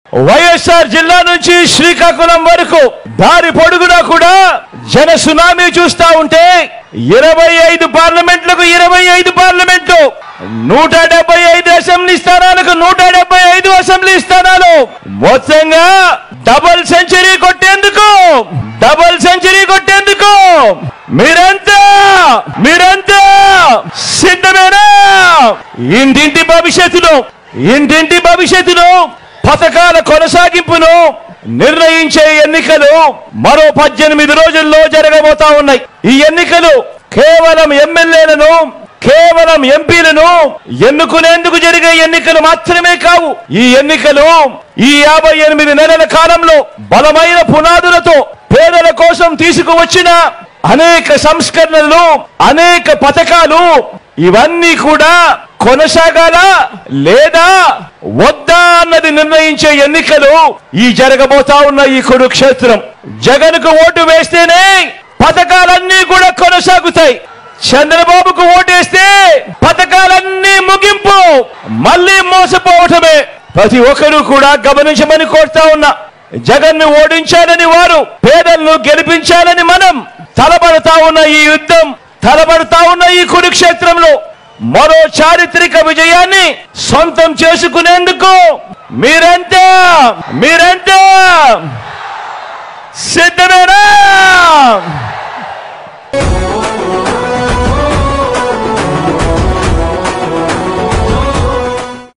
If you#Vote for#Jagan,all the schemes will continue.If you vote for Chandrababu,the schemes will end
జగన్‌కు ఓటేస్తేనే పథకాలన్నీ కొనసాగుతాయి. చంద్రబాబుకు ఓటేస్తే పథకాలన్నింటికి  ముగింపే.

-సీఎం వైయస్ జగన్

All the schemes will continue only if you vote for Jagan. If you vote for Chandrababu, all the schemes will end.